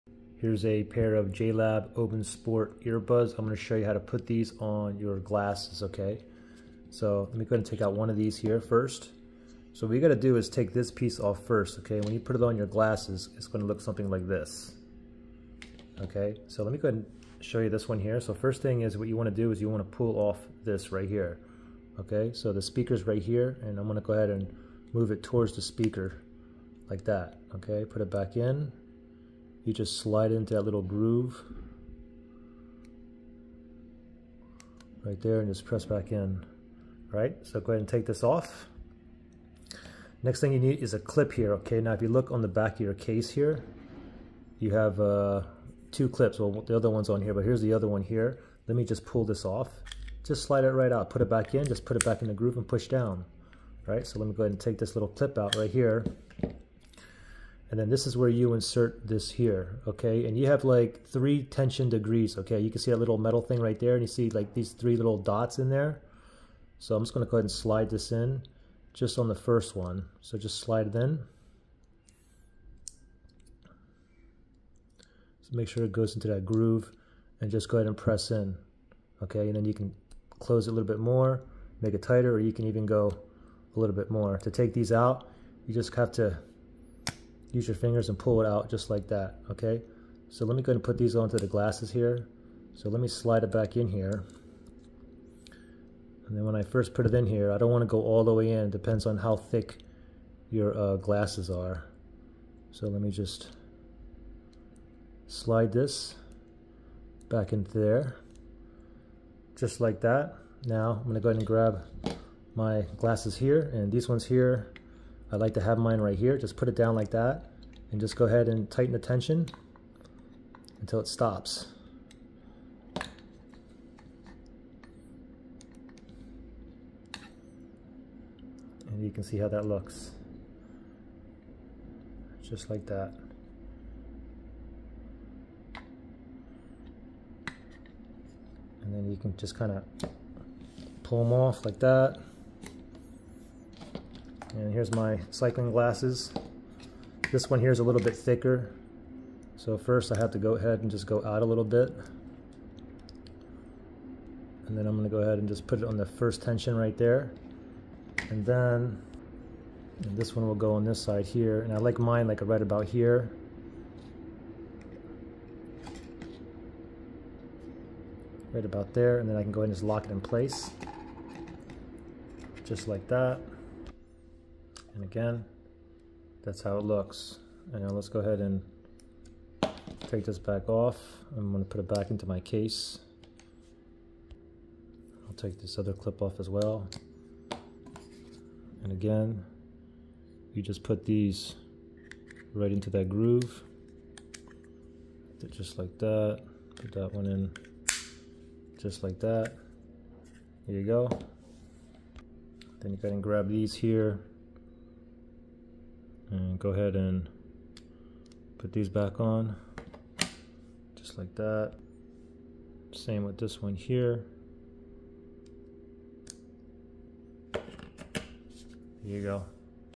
JLab Open Sport Earbuds: How to Attach to Glasses
I show you how to attach (put on, connect) a pair of JLab Open Sport earbuds to your glasses or sunglasses. I hope this helps.

JLab Open Sport Open-Ear Wireless Earbuds, Black, Flexible Earhook, Bluetooth Multipoint, Dual Connect, Safe Listening, Optional Glasses Clip, 24+ Hours Battery Life, Custom EQ Sound Settings